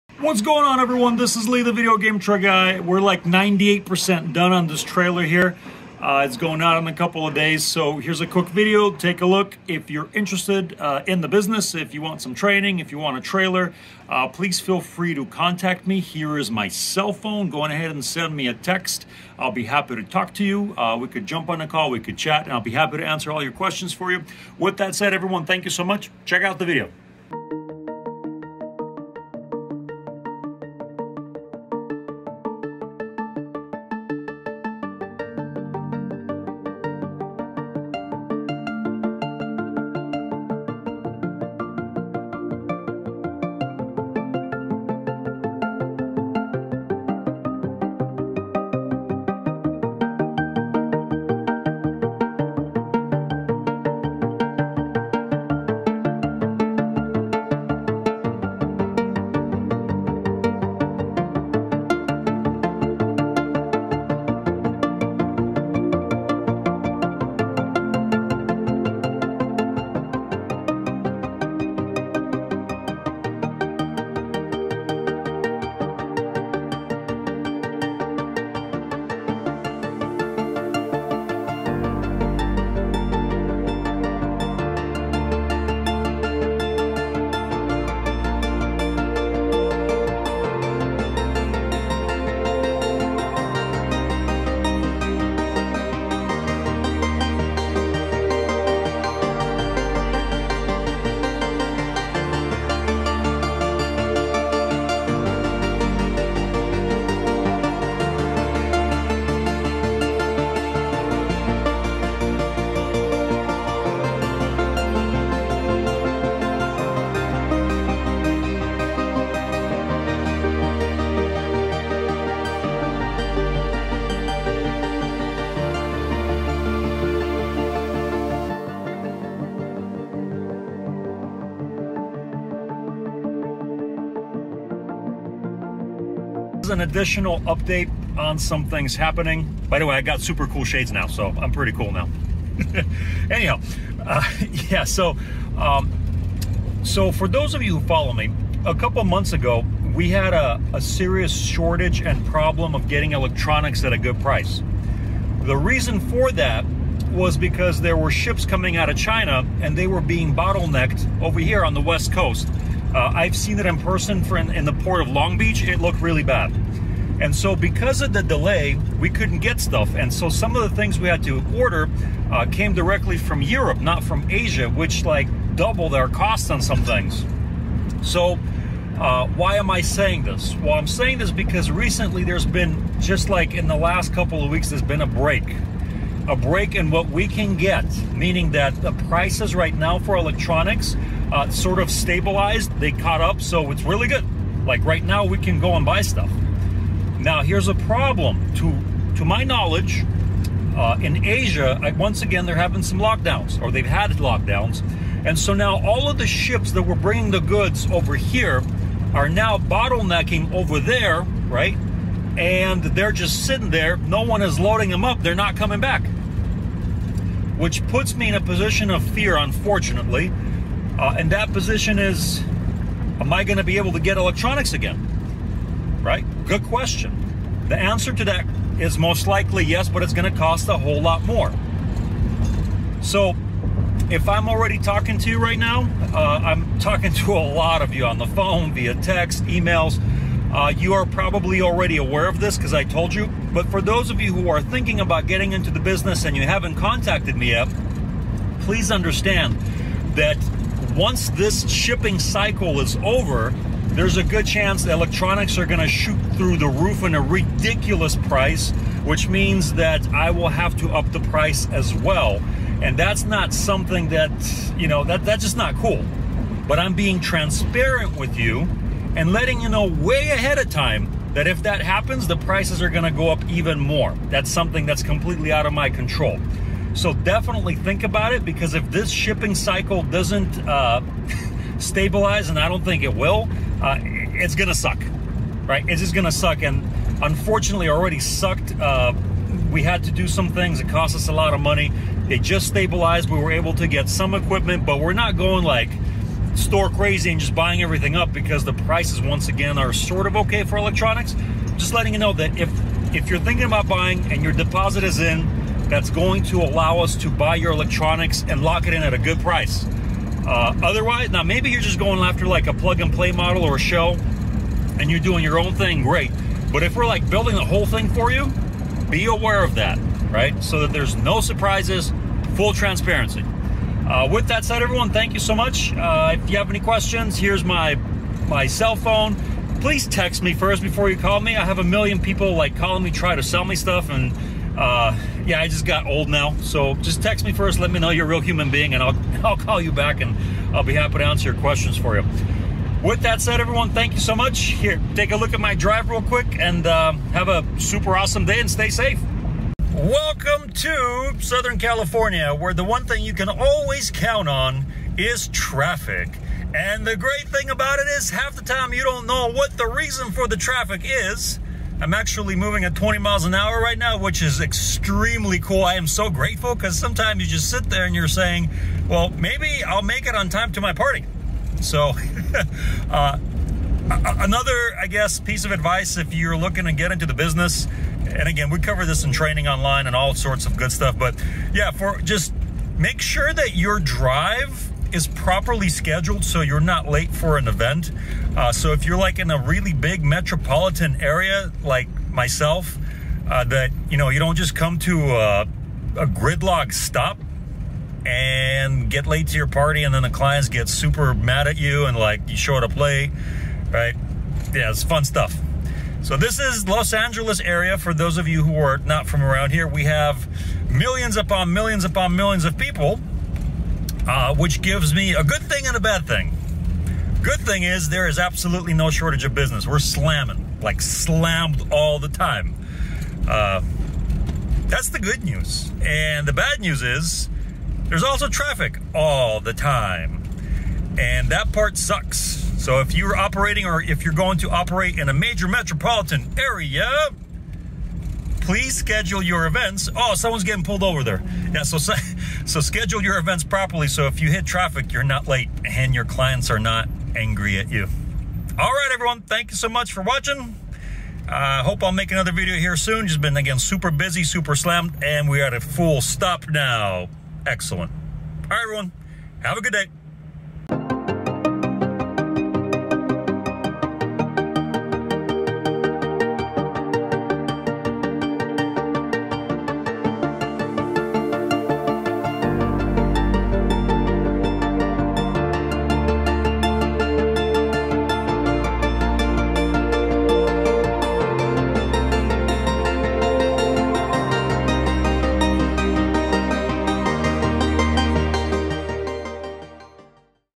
Video Game Truck Business VR Trailer 5-4-2022
Note: trailer options and pricing may not be current

In this video i show a trailer that is almost complete and talk about a potential problem with electronics in the very near guture.

Video game truck business
Virtual Reality business
VR Game Truck